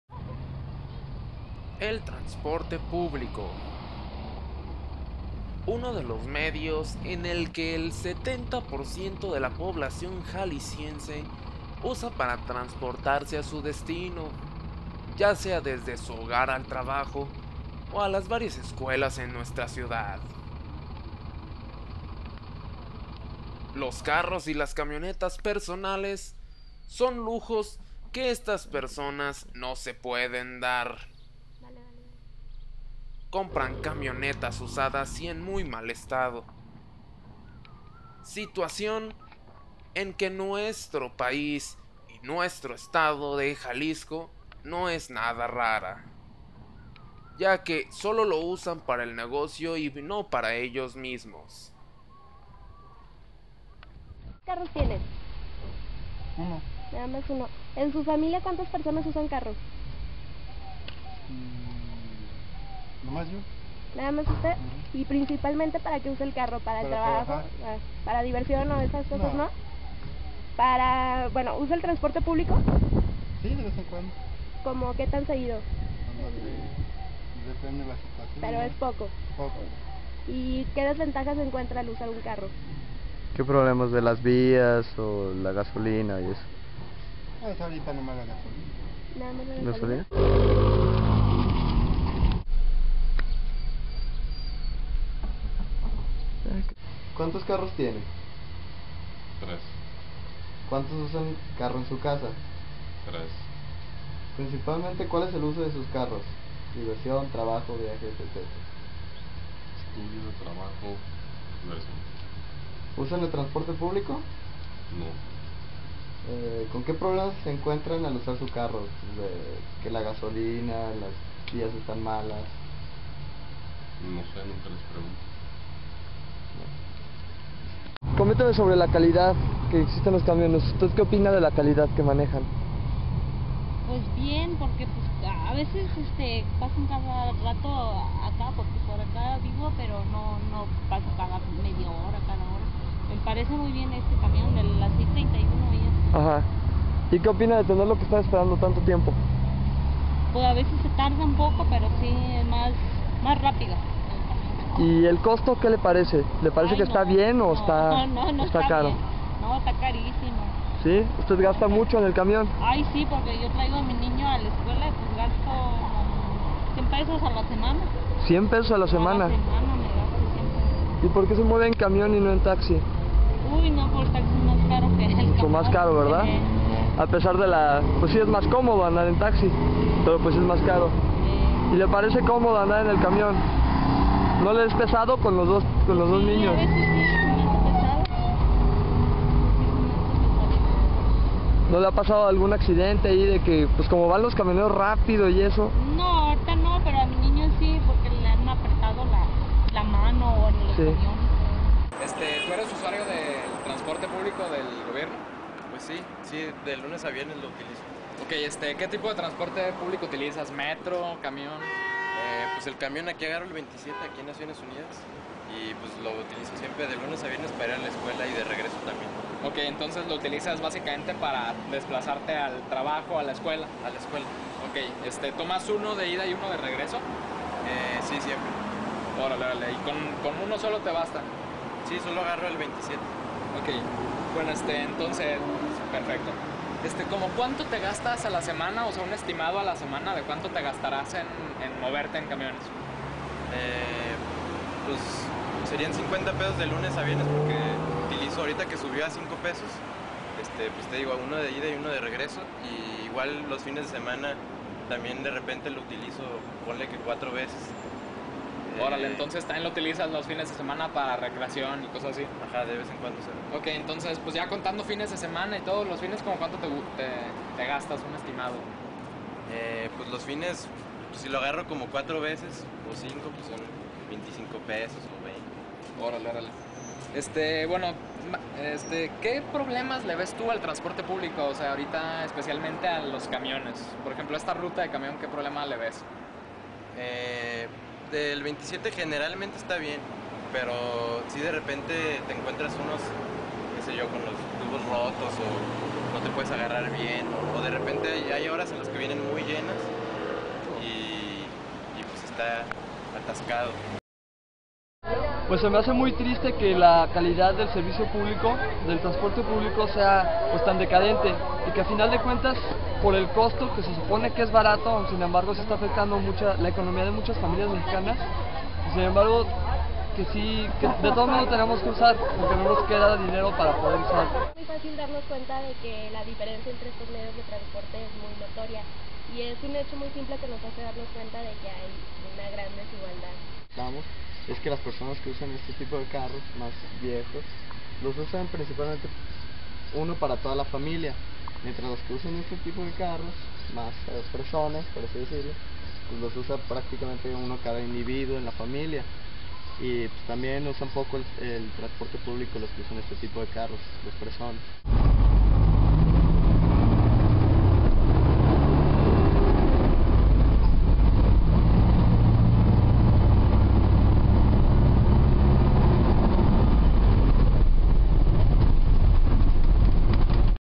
Transporte Publico GDL
Transporte publico, documental para la clase de mariza